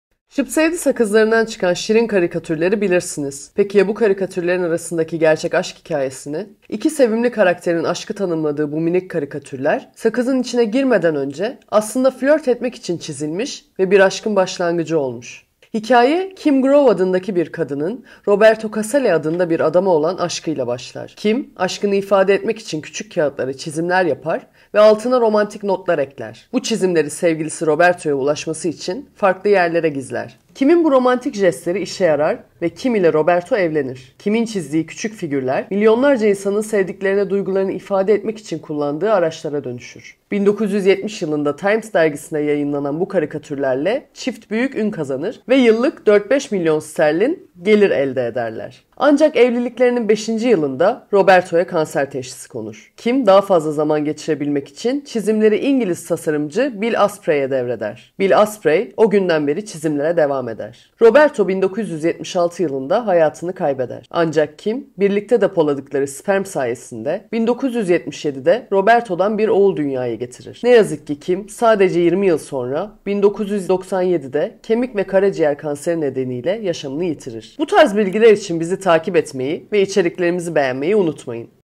Şıpsevdi sakızlarındaki karikatürlerin gerçek hayattaki hüzünlü hikayesi!
Şıpsevdi (Love is...) Sakızlarındaki Karikatürlerin Ardındaki Gerçek Aşk Hikayesi

Çocukken keyifle tükettiğimiz şıpsevdi sakızlarının gerçek hayattaki aşk hikayesini hiç duymuş muydunuz?